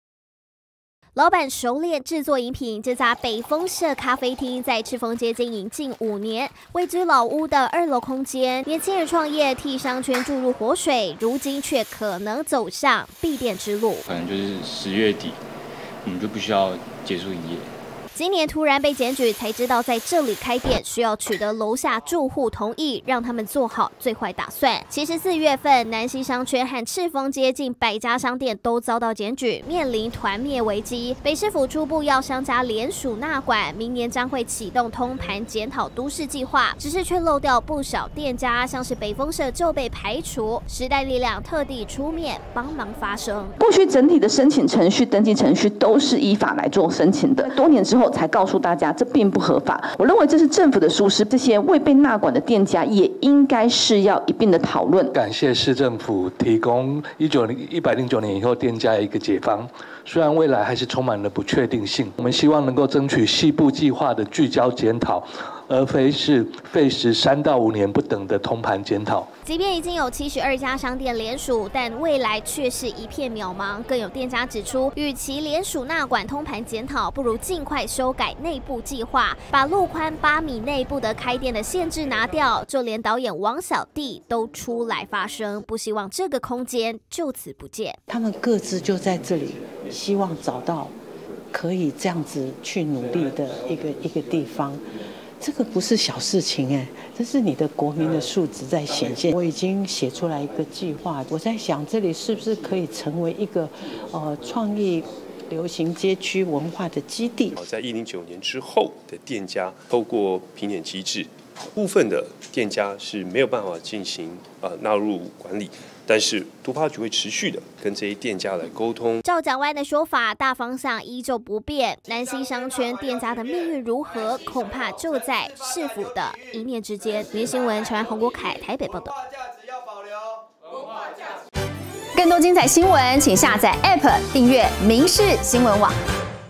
台北市府出手救南西商圈 時力痛批納管機制不全面－民視新聞
【民視即時新聞】生活中心／綜合報導台北市的南西商圈日前受到不明檢舉，近百家商店面臨團滅危機，北市府祭出連署納管機制，也喊話要明年啟動都市計畫通盤檢討，但有商家抱怨前途渺茫，今天時代力量也舉行記者會，出面替商家發聲，質疑蔣市府全盤檢討曠日廢時，抨擊納管評點機制根本不夠全面。